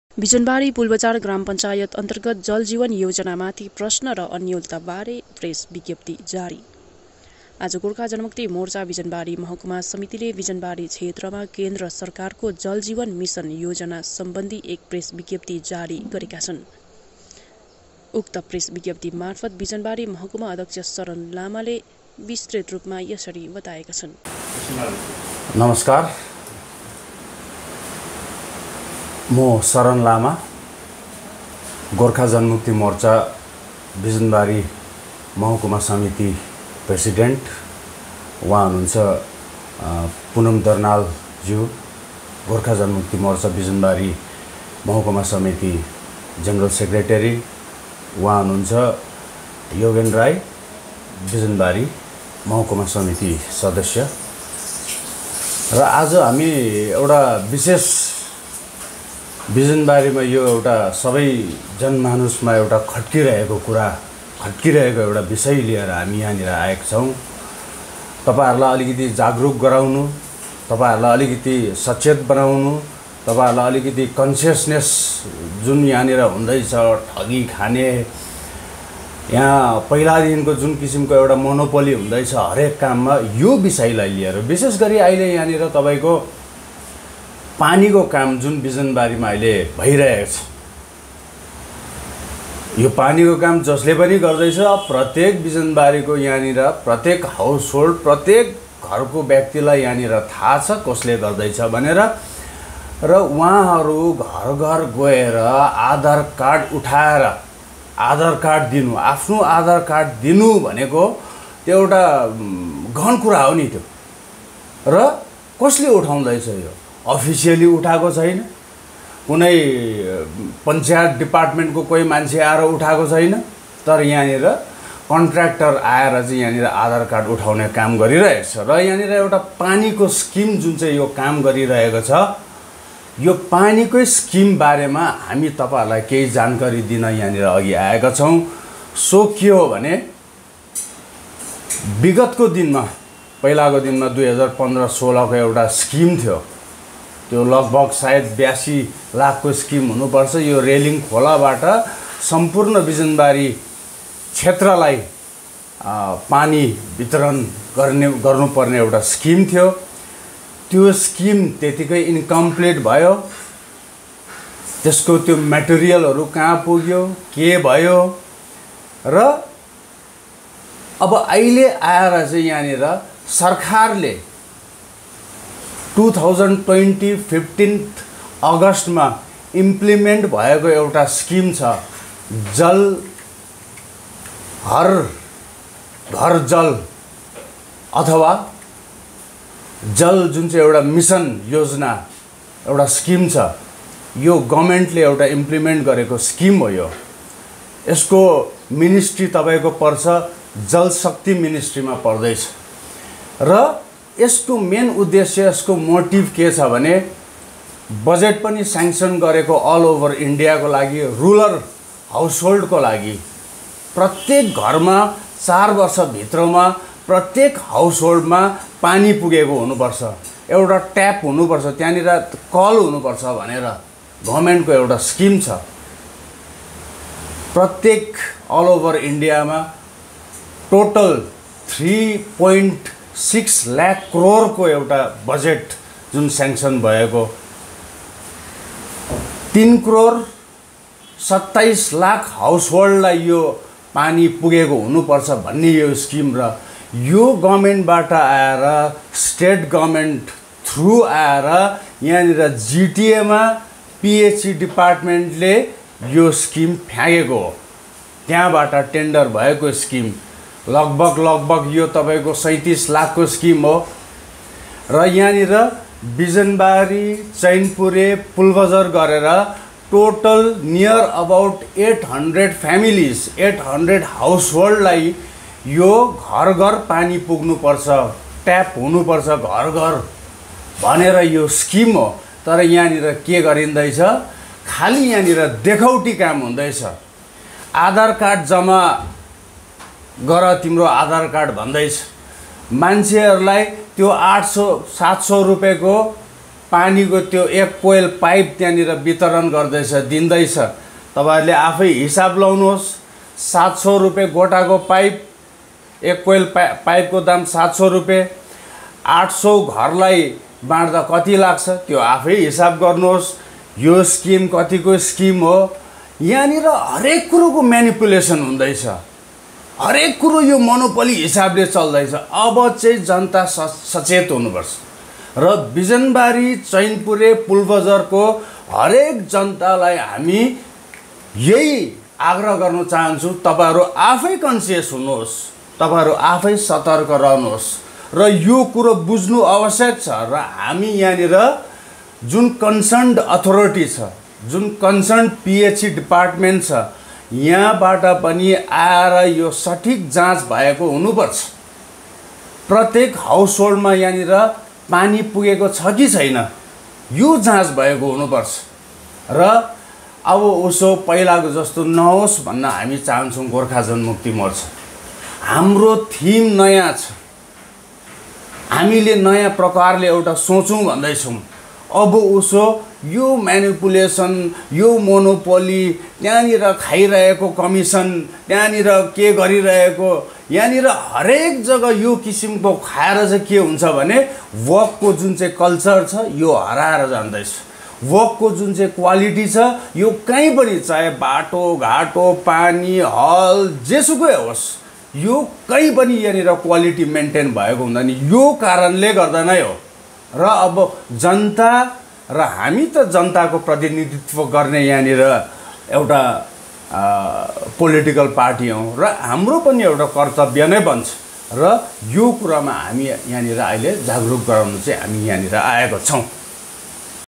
बिजनबारी-पूलबजार ग्राम पंचायत अन्तर्गत जल जीवन योजना माथि प्रश्न र अन्योलता बारे प्रेस विज्ञप्ति ।
आज दिनांक ११/१०/२०२१ को दिन गोर्खा जनमुक्ति मोर्चा बिजनबारी महकुमा समितिले बिजनबारी क्षेत्रमा  केन्द्र सरकारको जल जीवन मिसन योजना सम्बन्धि एक प्रेस विज्ञप्ति जारी गरेको छ भने उक्त विषयमा  बिजनबारी महकुमा अध्यक्ष शरण लामाले विस्तृत रूपमा यसरी बताएका छन् ।